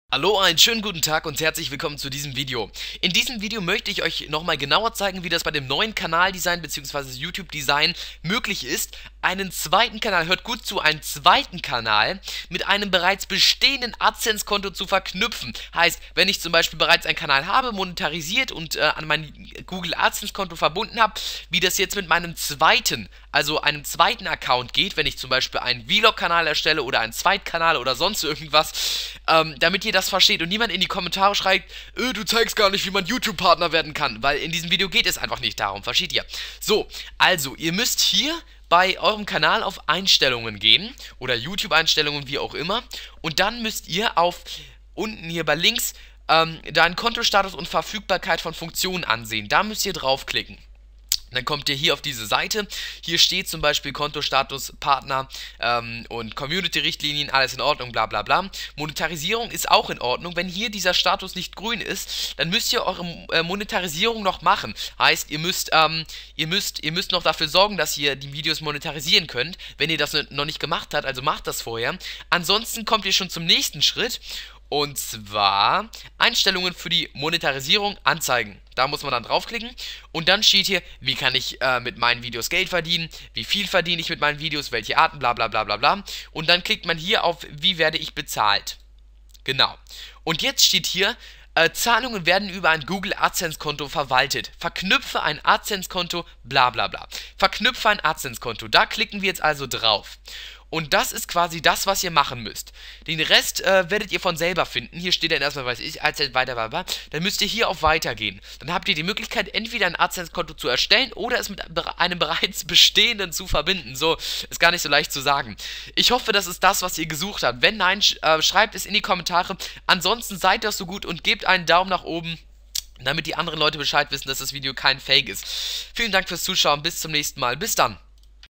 2 YOUTUBEKANÄLE MIT ADSENSE KONTO VERKNÜPFEN !!! [Neues Design 2013]
Guten Tag Leute!
In diesem Video werde ich euch zeigen, wie es möglich ist, zwei verschiedene Kanäle wie z.B. MV10 und MVSynchros mit einem bereits bestehendem Adsense Konto zu verknüpfen, nur eben jetzt mit dem neuen Design (da ich schon ein altes Video hatte)

MV10 - Hier liegt die Lösung!